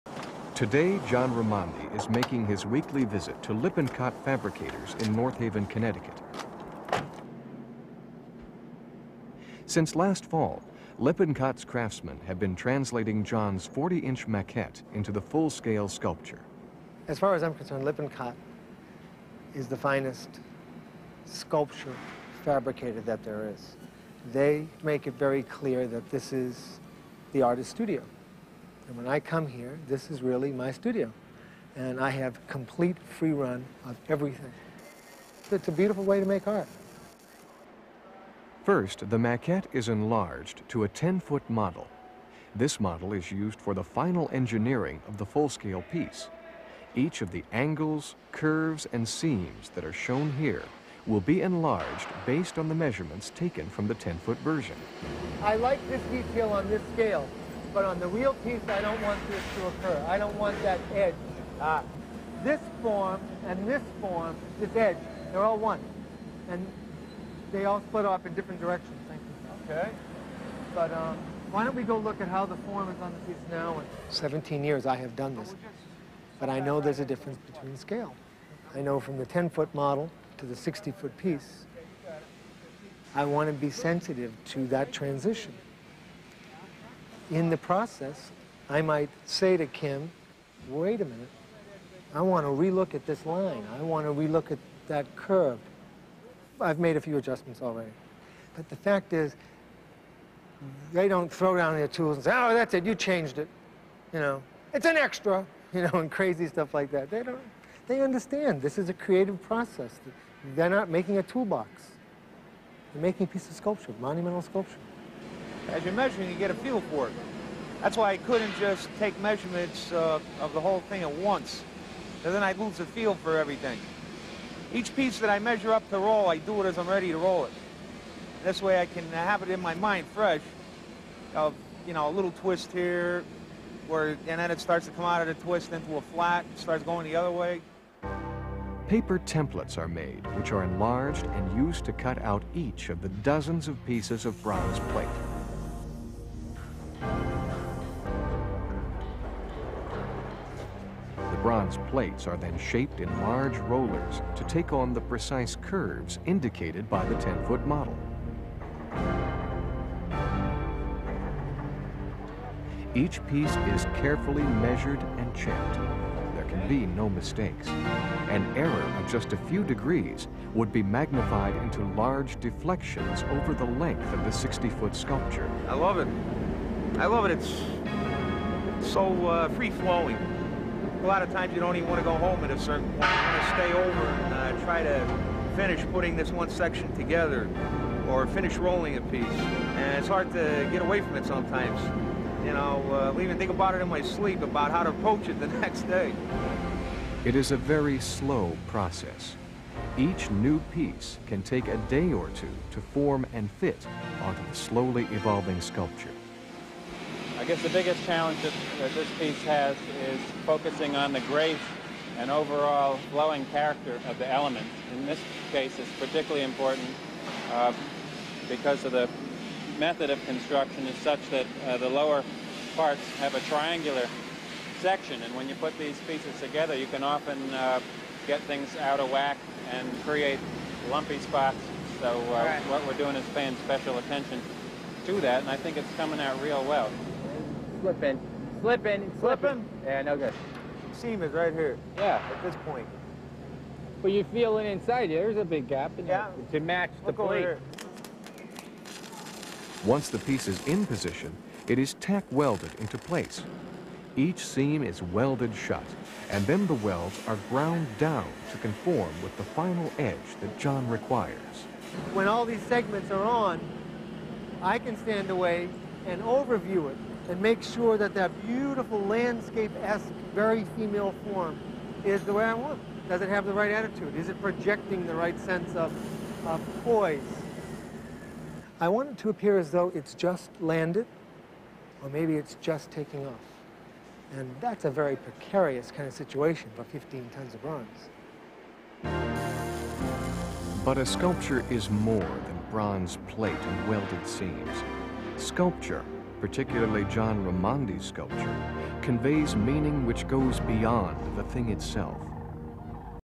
Danc of the Cranes -- Fabrication
John Raimondi introduces us to the fabrication process.